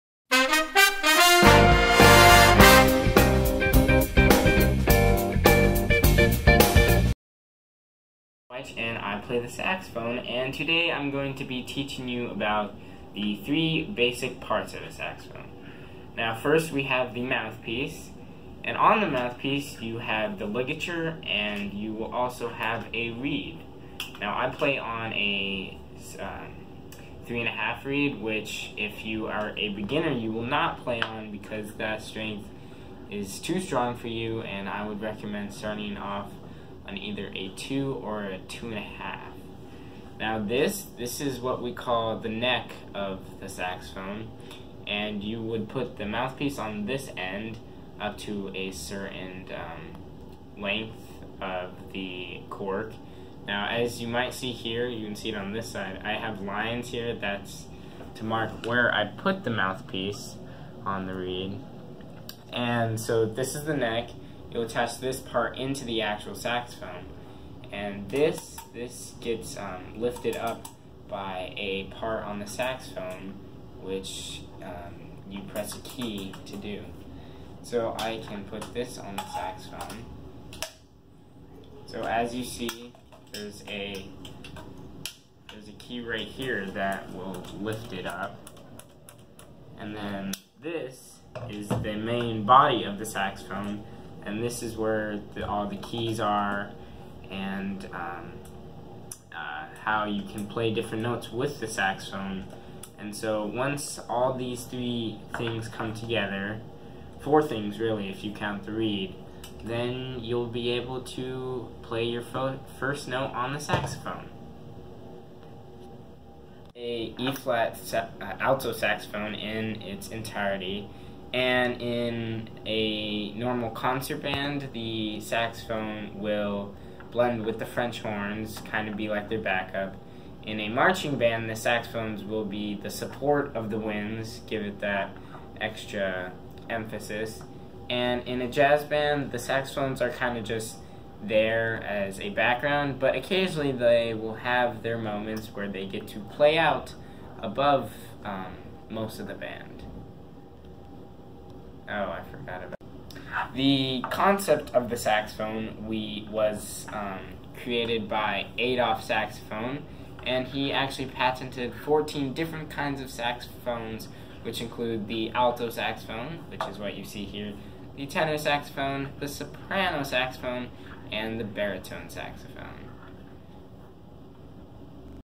Sax Series: Intro to the Alto Saxophone!
Welcome to our first video! Today we have our guest Paul giving a brief introduction to beginners on the basics of the Eb Alto Saxophone.